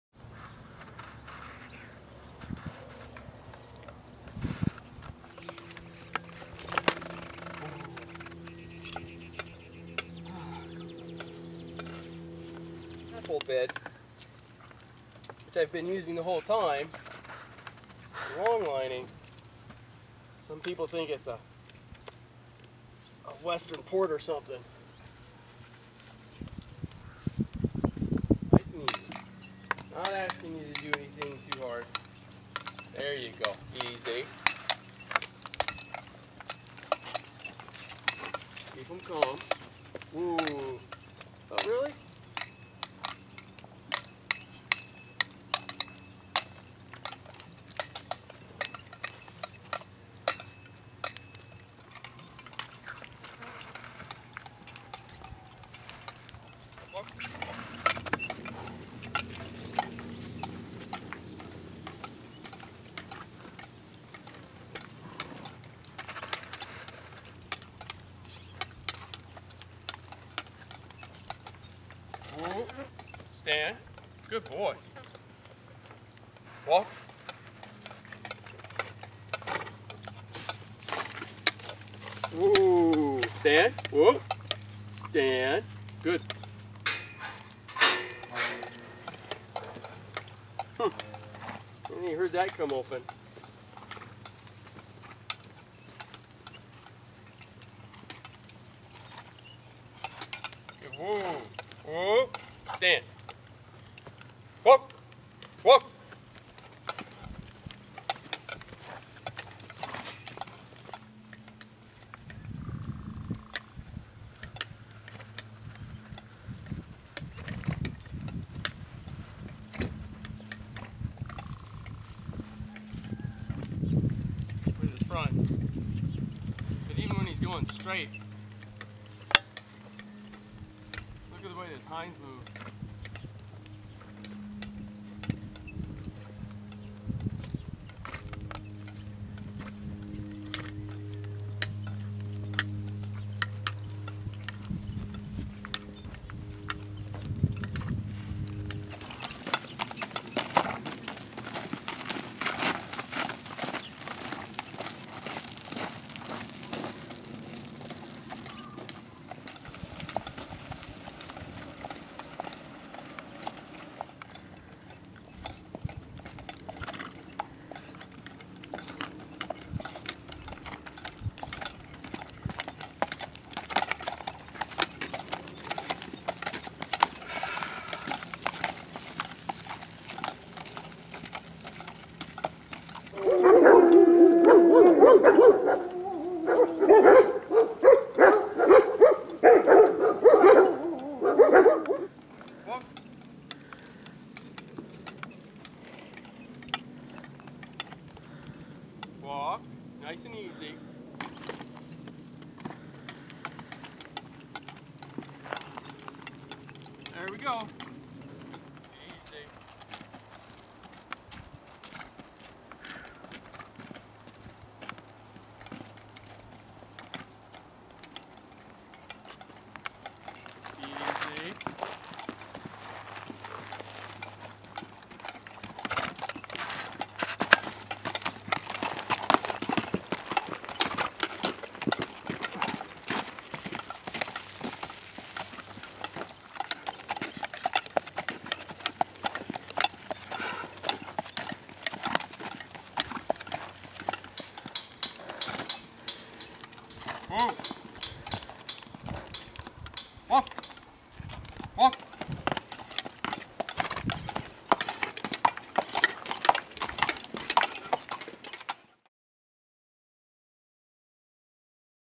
Long-lining a horse on trail
Marley did much better today in his session with Tim. He worked on long-lining in arena, then pasture and eventually out on trail. He was calm and went without any incident or rearing. He seems to have his “good days” and then his “bad days”. He did really well long-lining out on trail today. Very happy with his progress.